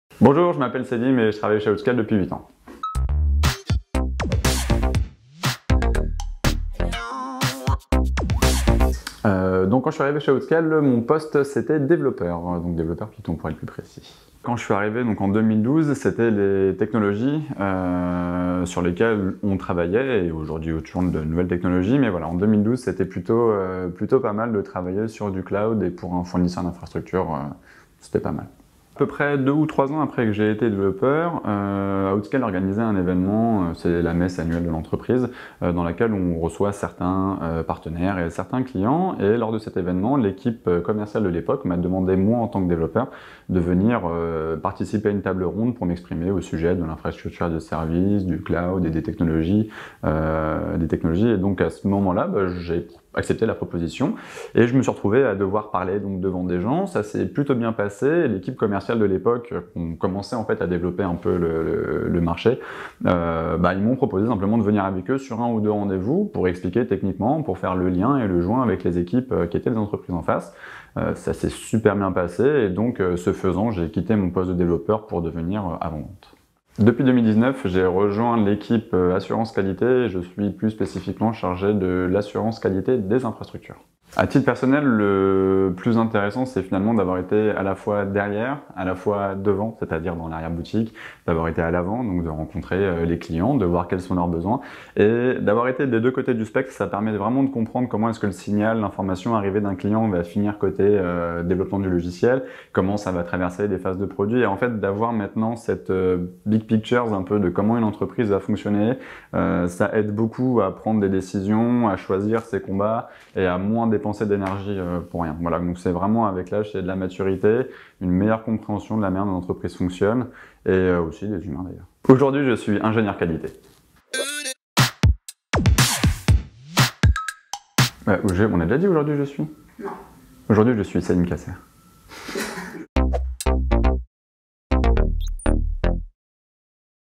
Spitch My Job Story avec Selim
Rencontrez Selim ! Développeur logiciel, Avant-Vente, puis Ingénieur Qualité chargé de l'assurance qualité des infrastructures... Aujourd'hui, Selim vous raconte son parcours exceptionnel chez 3DS OUTSCALE et ce qu'il trouve de plus intéressant dans son expérience de mobilité interne

▼ PLUS D'INFORMATIONS ▼